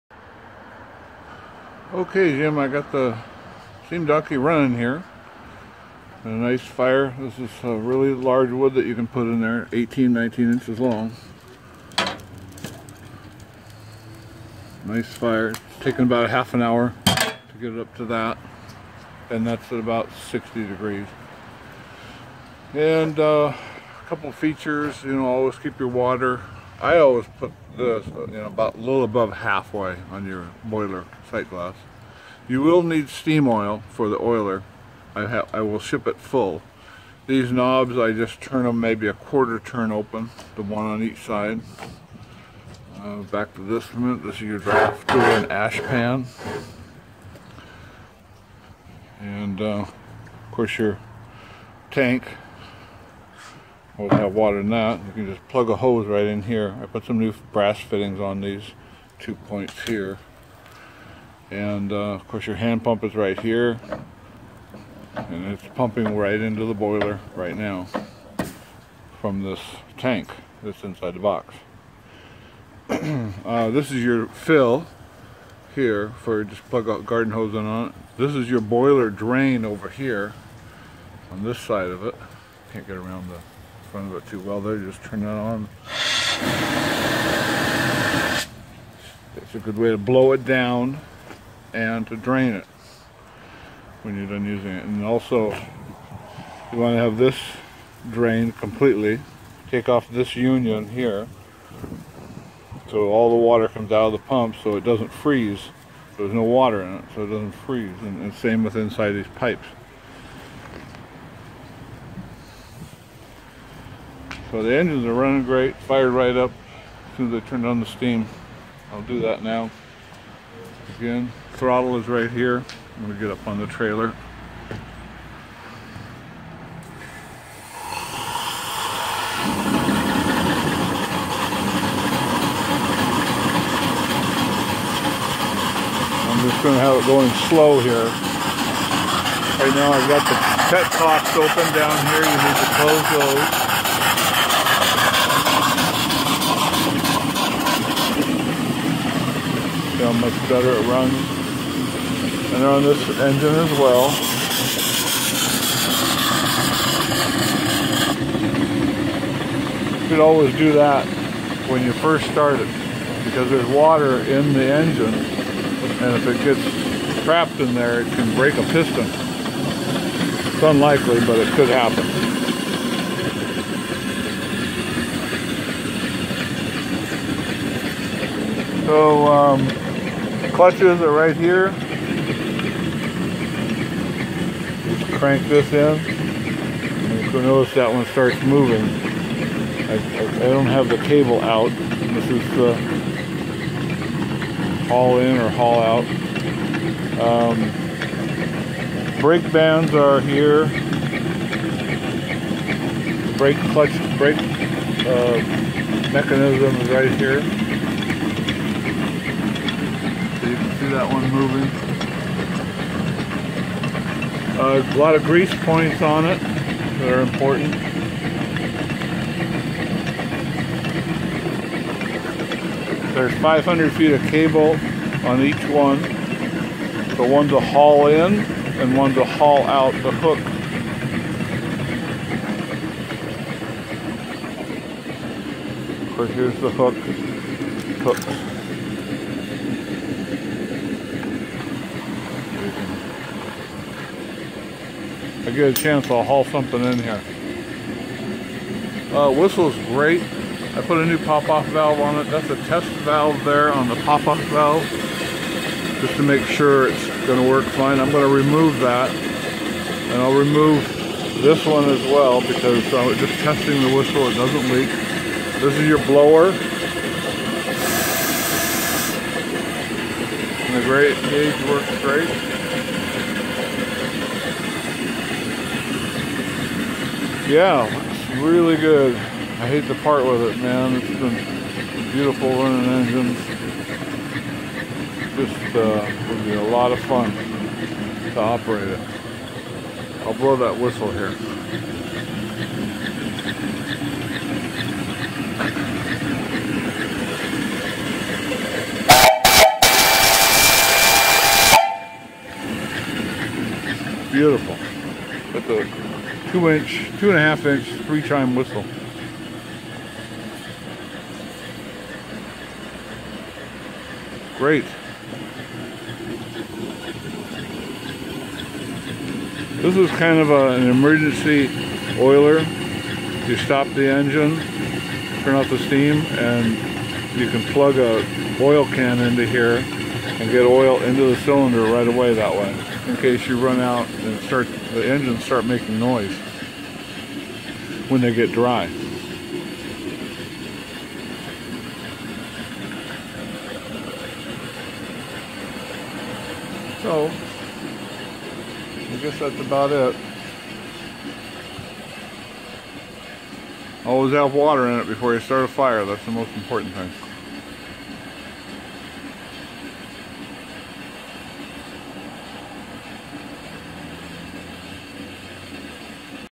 Steam Donkey Boiler Whistle Water Pump
This is the third Steam Donkey that I have built and the last. Measuring 12' long x 42" wide x 72" high to the top of the stack. The donkey includes two engines and cross slides. Also included are two 6-inch diameter by 1-inch steel crank disks, bronze eccentrics, cast iron piston and rings, and all moving parts for the engines. The two engines run on just 25 lbs. of steam pressure. The bore is 2.75 inches with a 4-inch stroke. It also features a 20" dia. x 42"tall boiler that is 3/8-inch thick. There are 22 - 1-1/2 copper tubes rolled in. A Swift oiler for both engines, a pop off valve set at 100 lbs. and two 10-inch spool drums that hold 500 feet of 3/16 steel cable on each drum. Oak clutches, braking system and ratchet-and-pawl on each spool.